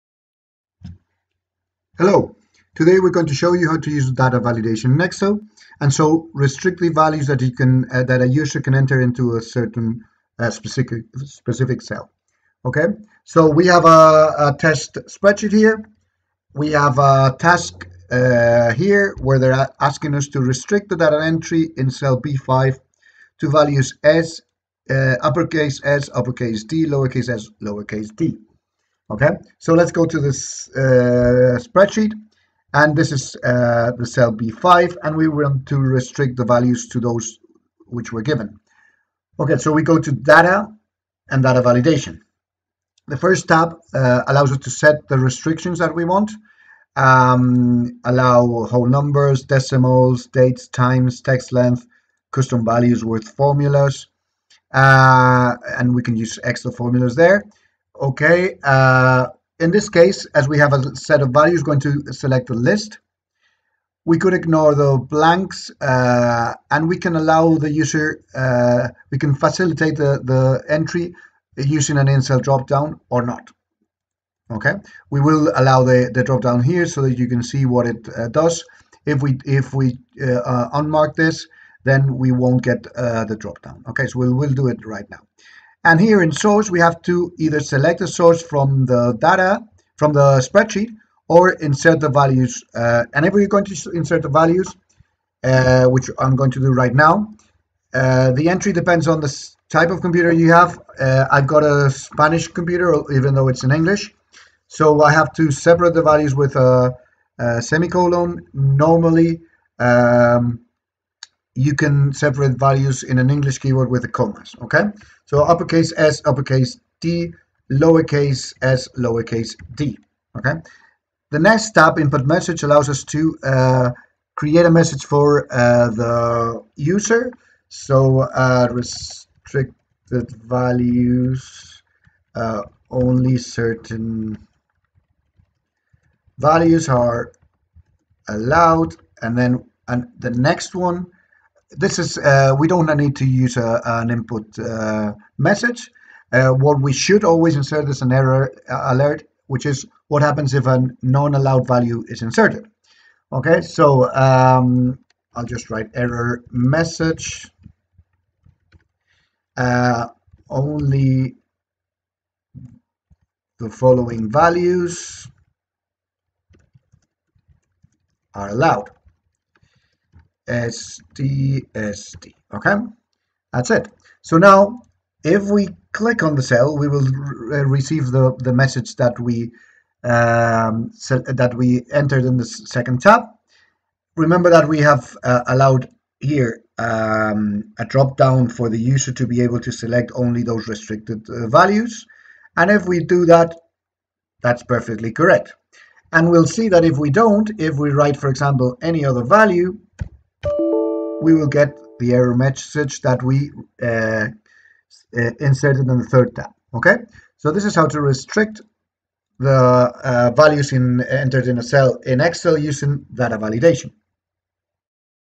Excel: data validation. How to restrict entries to a cell a certain values or types of values.
Today we explain how to perform data validation in Excel and restrict the entry of values into a cell.